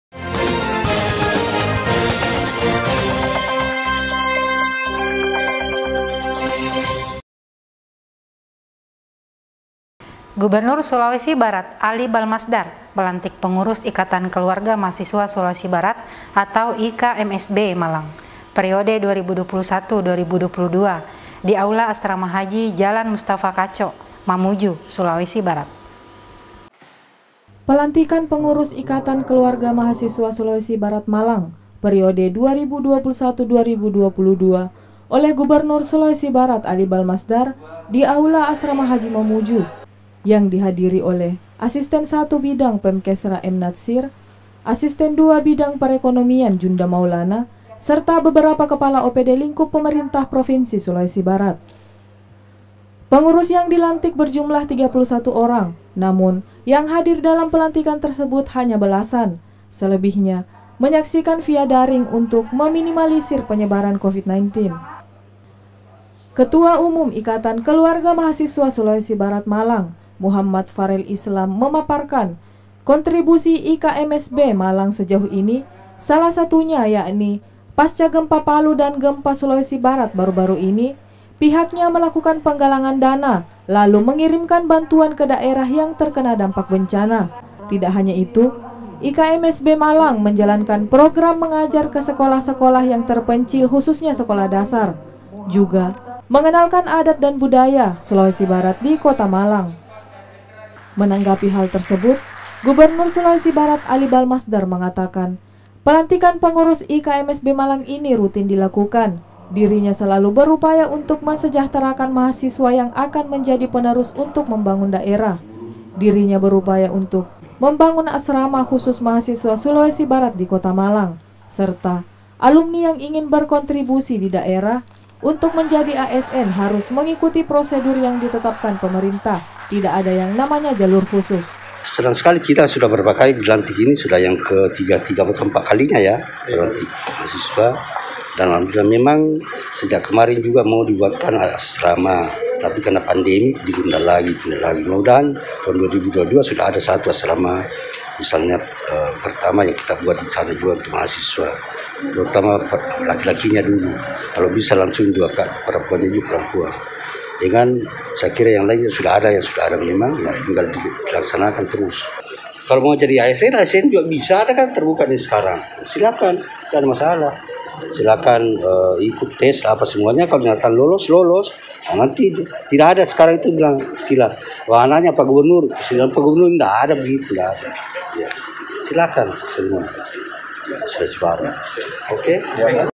GUBERNUR SULBAR LANTIK PENGURUS IKMSB MALANG
PENGURUS YANG DILANTIK BERJUMLAH 31 ORANG, NAMUN YANG HADIR DALAM PELANTIKAN TERSEBUT HANYA BELASAN, SELEBIHNYA MENYAKSIKAN VIA DARING UNTUK MEMINIMALISIR PENYEBARAN COVID-19